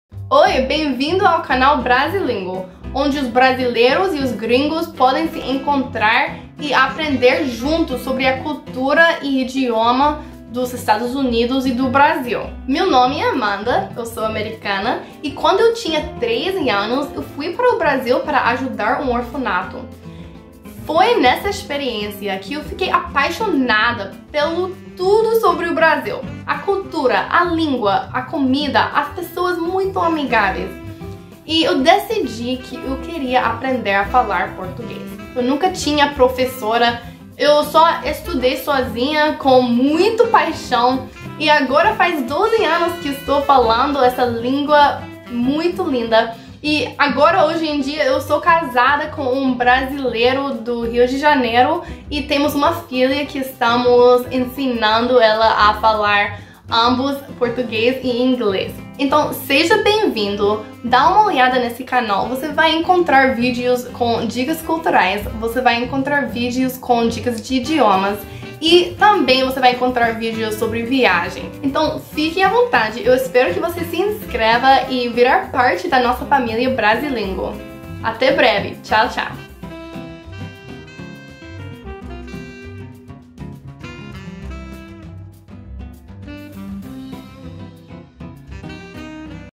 Bem-vindo ao Brazilingo! (Português)
Bem-vindo ao canal Brazilingo! Vai achar vídeos com dicas de idiomas em inglês e português e também vídeos sobre cultura nos EUA e no Brasil.